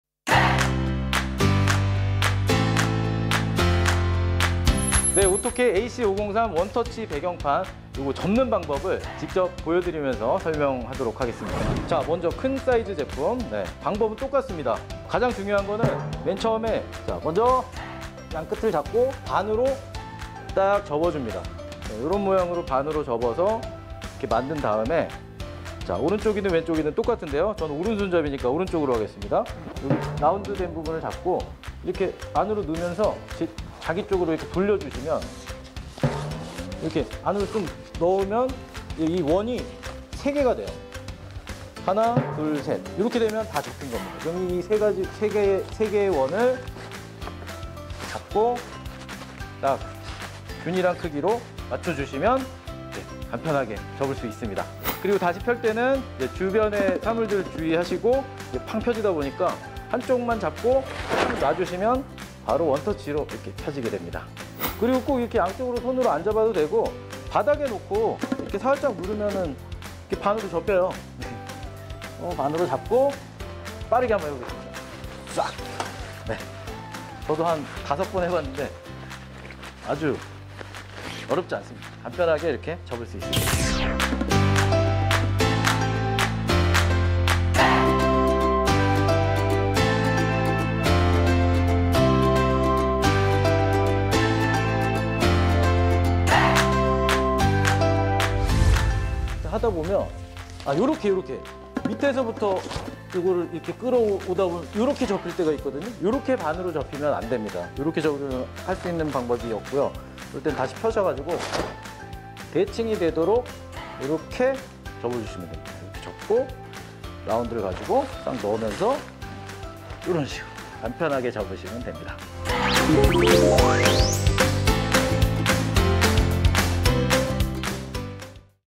오토케 AC503 원터치 설치 배경판 패널 (크로마키 그린 블루 블랙 화이트) 쉽게 접고 펴는 방법!
원터치로 간편하게 설치하여 사용할 수 있는 배경판 AC503 시리즈를 쉽게 접고 펴는 방법! 지금 동영상을 시청하며 따라해보세요!

AC503 원터치 설치 접이식 배경판 시리즈